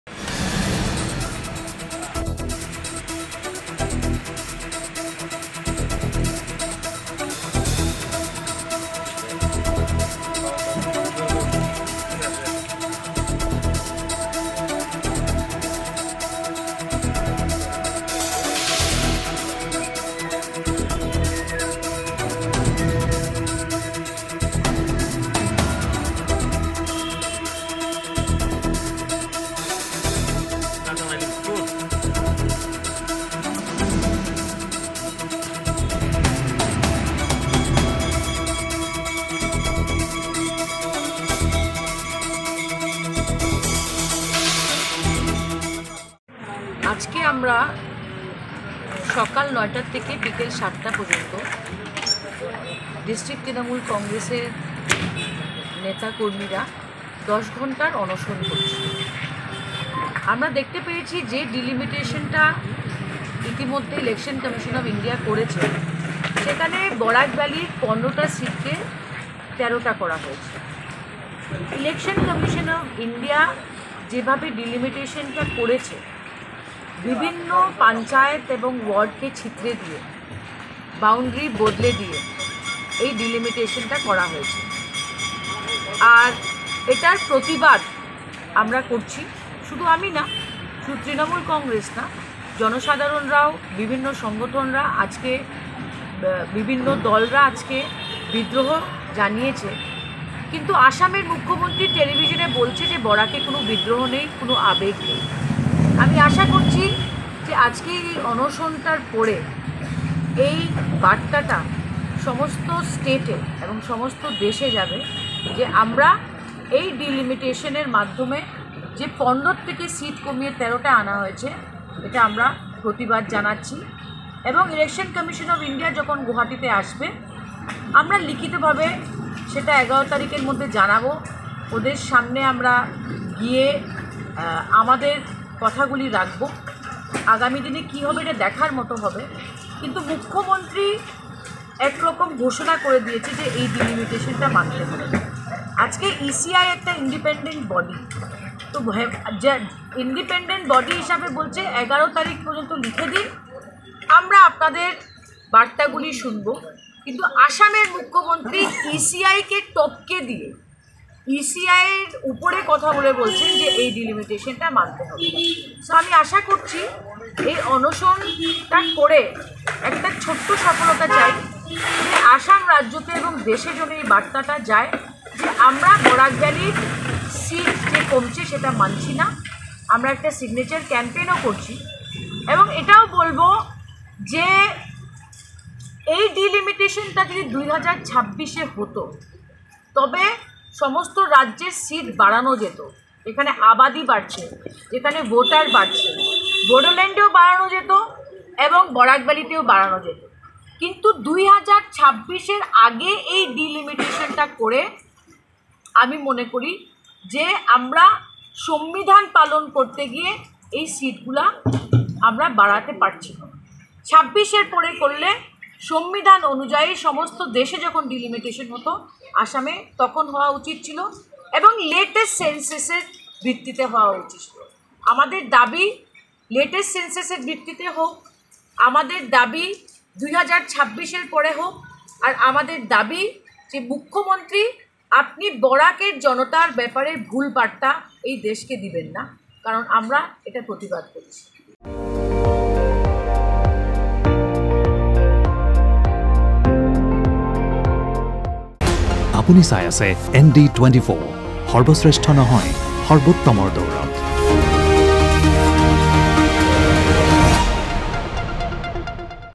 Assam Delimition Protest : ১০ ঘণ্টীয়া অনশনত বহিল TMCৰ ৰাজ্যসভাৰ সাংসদ সুস্মিতা দেৱ
বৰাকত সমষ্টি পুনঃনিৰ্ধাৰণক লৈ অব্যাহত প্ৰতিবাদ৷ ১০ ঘণ্টীয়া অনশনত বহিল TMCৰ ৰাজ্যসভাৰ সাংসদ সুস্মিতা দেৱ৷ প্ৰতিবাদ সাব্যস্ত কৰি কি ক’লে সাংসদগৰাকীয়ে? চাওক...